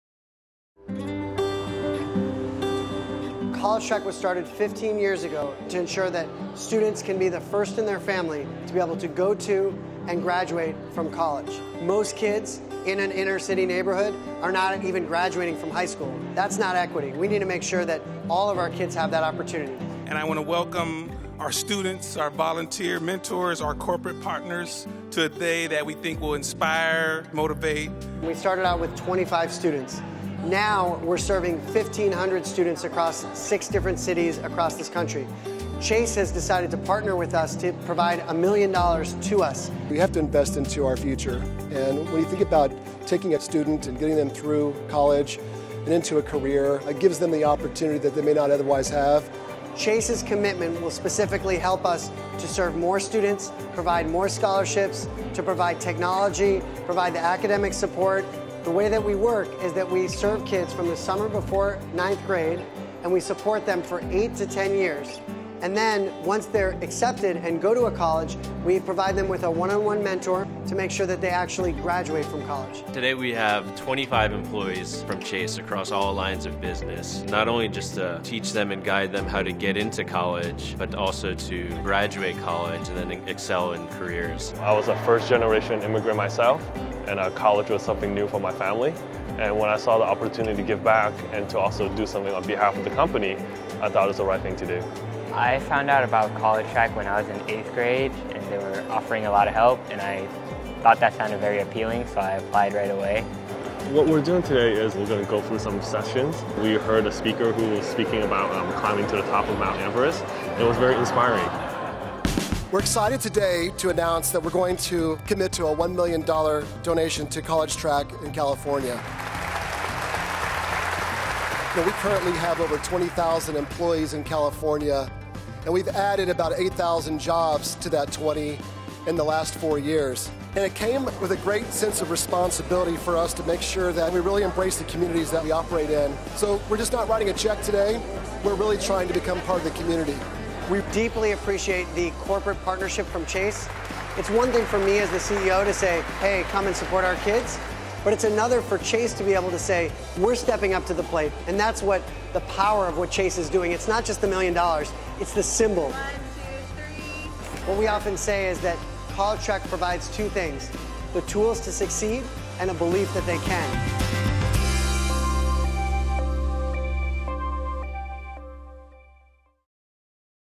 Empowering Students: College Track San Francisco Day
In this video produced by Chase, see how the bank's $1 million donation to College Track is helping students in California achieve their dreams of a college degree.

College Track is a college completion program that actively engages low-income students from the summer before 9th grade through college graduation and provides them with the academic, social and financial resources necessary to succeed. College Track started in 1997 in East Palo Alto, CA with 27 high school freshmen from Carlmont High School. Since then, the program has grown to serve over 1,400 students in six cities: East Palo Alto, Oakland, San Francisco, Los Angeles, New Orleans and Aurora, CO.